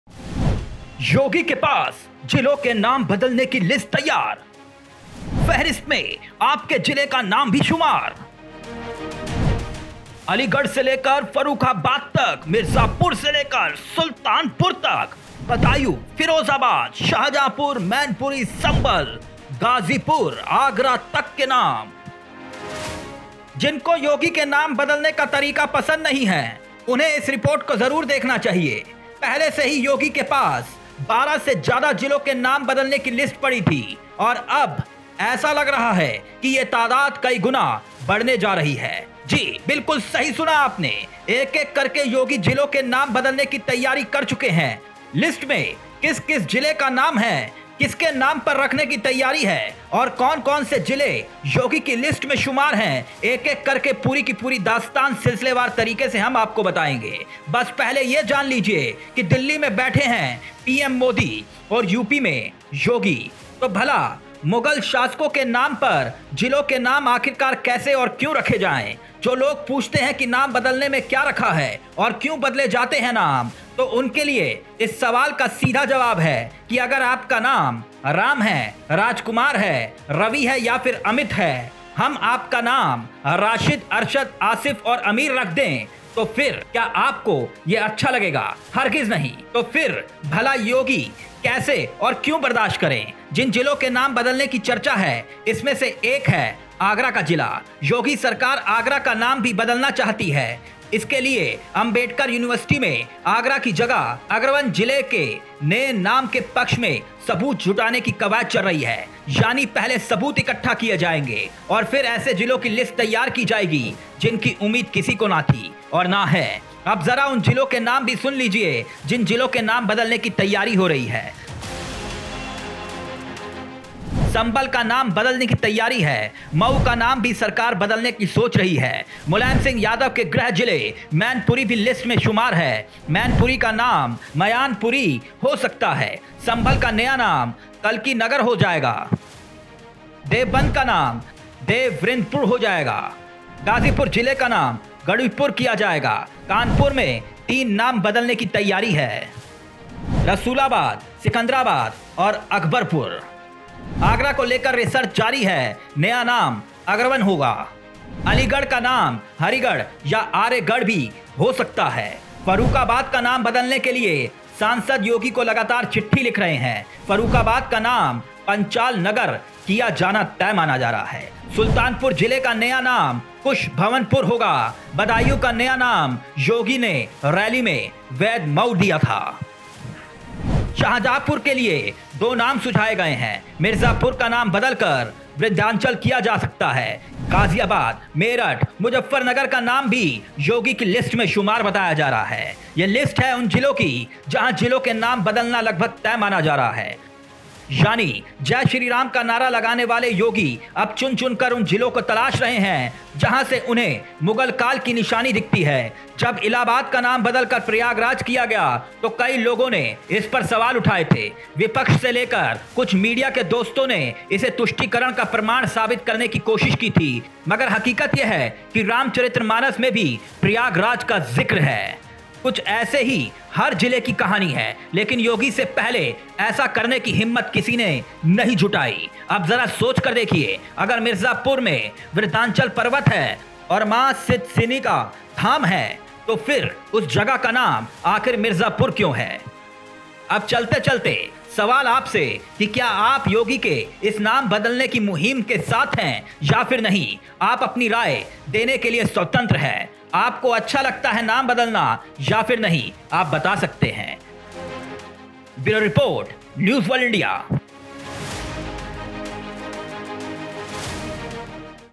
Yogi इन 12 शहरों के बदलेंगे नाम, लिस्ट देखी क्या ? आधे UP का बदलने वाला नाम ?
Yogi govt 2.0 : UP Government mulling to rename 12 districts,
संभल का नाम बदला जाएगा
मऊ का नाम भी सरकार बदल सकती है
मुलायम सिंह यादव के गृह जिले मैनपुरी भी लिस्ट में शुमार है
मैनपुरी का नाम मयानपुरी हो सकता है
संभल का नया नाम कल्कि नगर हो जाएगा
देवबंद का नाम देव वृंदपुर हो जाएगा
गाजीपुर जिले का नाम गढ़ीपुर किया जाएगा
कानपुर में 3 नाम बदले जाएंगे...
रसूलाबाद, सिकंदराबाद और अकबरपुर
आगरा को लेकर रिसर्च जारी है नया नाम अग्रवन होगा !
अलीगढ़ का नाम हरिगढ़ या आर्यगढ़ कभी भी हो सकता है
फर्रूखाबाद का नाम बदले के लिए सांसद योगी को लगातार चिट्ठी लिख रहे हैं
फर्रूखाबाद का नाम पांचाल नगर होना तय है
सुलतानपुर जिले का नया नाम कुश भवनपुर होगा
बदायूं को नया नाम योगी ने रैली में वेदमऊ दिया था
शाहजहांपुर के लिए दो नाम सुझाए गए है
मिर्जापुर का नाम बदलकर विध्याचंल किया जा सकता है
गाजियाबाद...मेरठ...मुज्जफरनगर का नाम भी योगी बदल सकते हैं
.ये लिस्ट हैं उन जिलों का जहां जिलो का नाम बदला जा सकता है